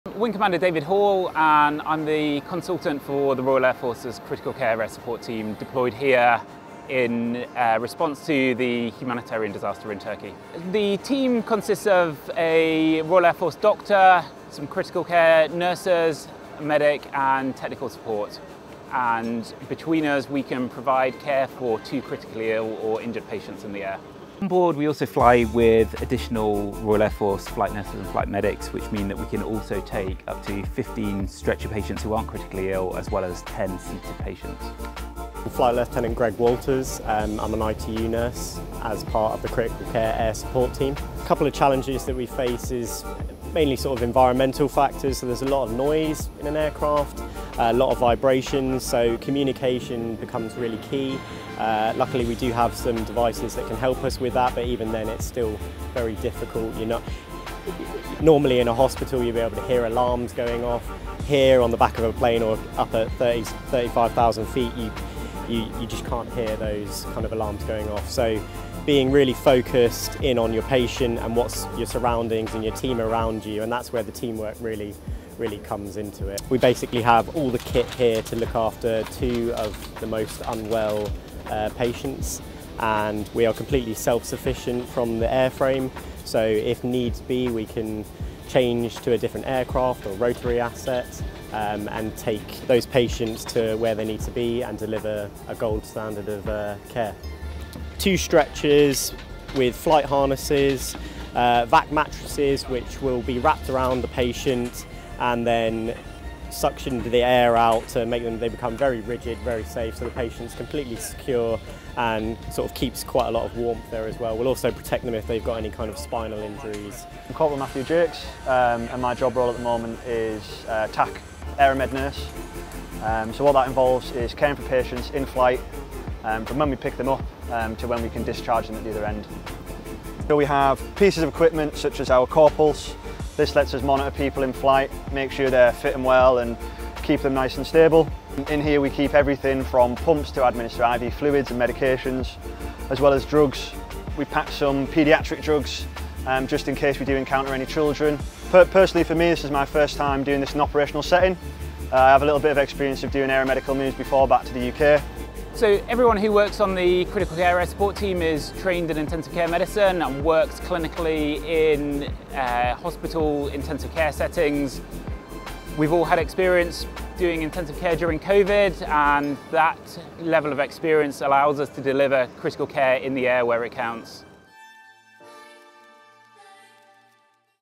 RAF Tactical Medical Wing support to Turkey earthquake victims
RAF Medical specialists provided urgent medical assistance to victims of the earthquake in Turkey.

The world’s first independent Air Force.